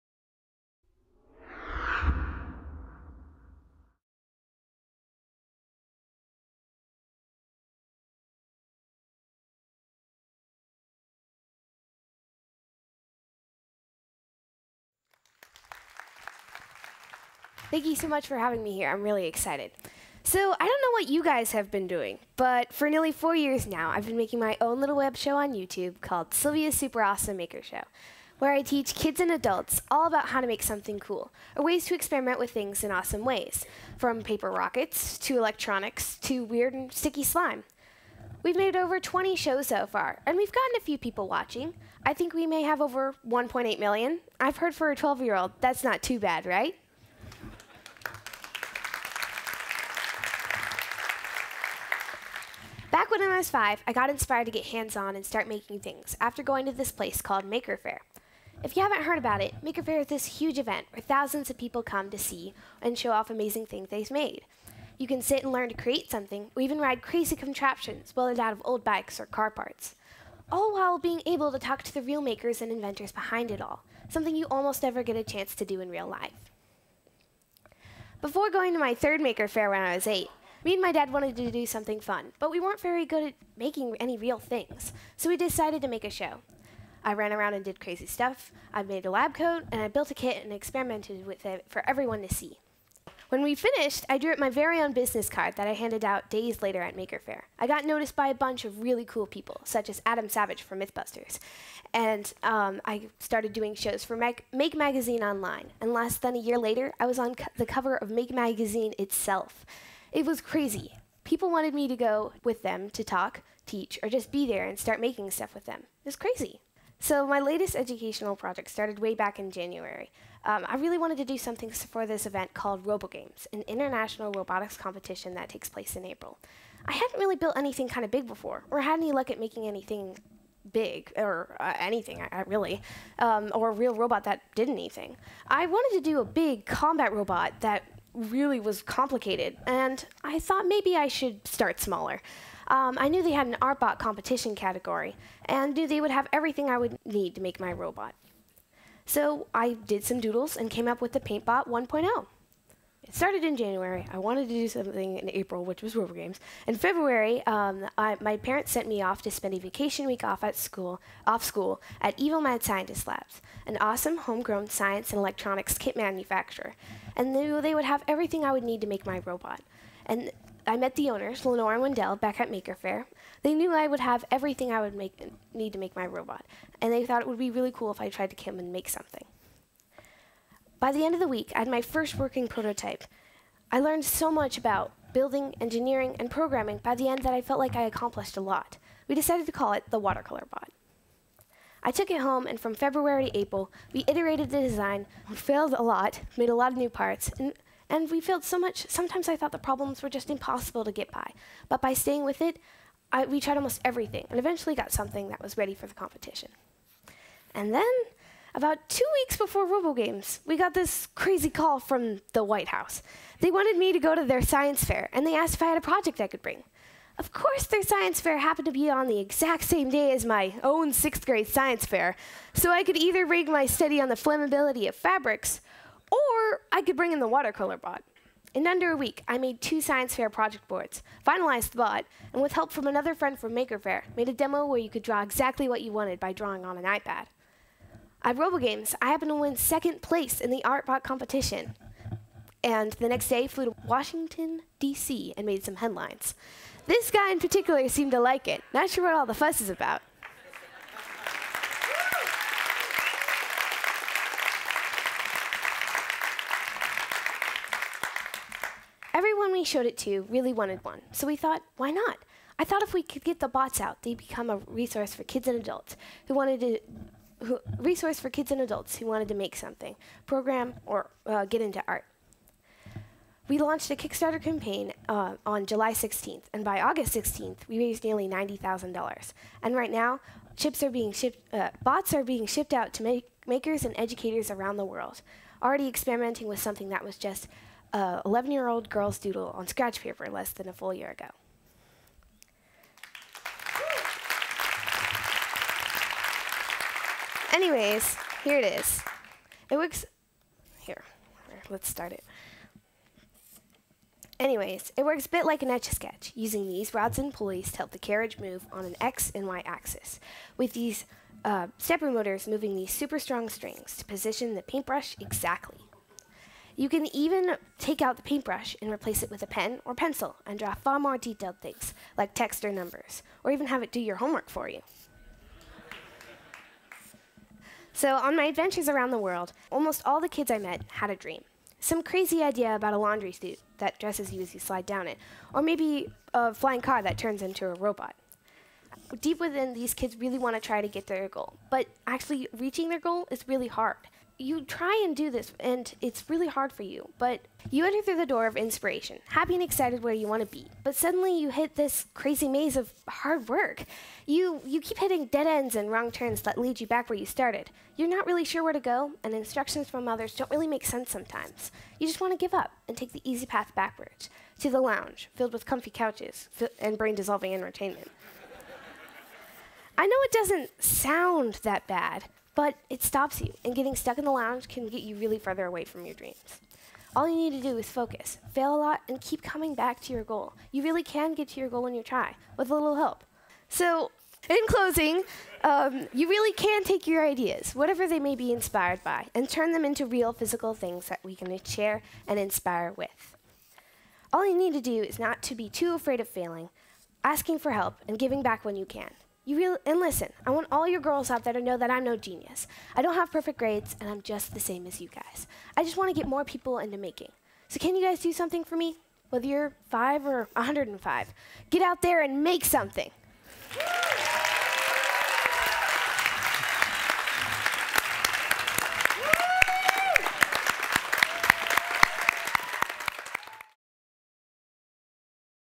Go out there and make something! Sylvia Todd at TEDxSanDiego 2013
Sylvia Todd, the girl behind the 'Sylvia's Super-Awesome Maker Show,' is only 12 years old but has over one million followers -- kids and adults alike. On her show Todd gets herself busy with 'everything cool and worth making' -- like an Arduino pendant that flashes to your heartbeat. Todd is as comfortable with a soldering iron and a hot-glue gun as she is with a microphone and a camera. Her show helps us realize that today's kids are enormously capable digital natives, with unlimited potential.